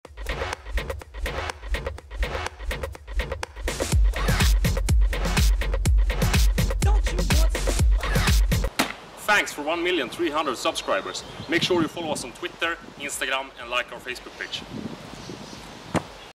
Impression of SkillTwins by Gurra
Gurra makes an impression of SkillTwins.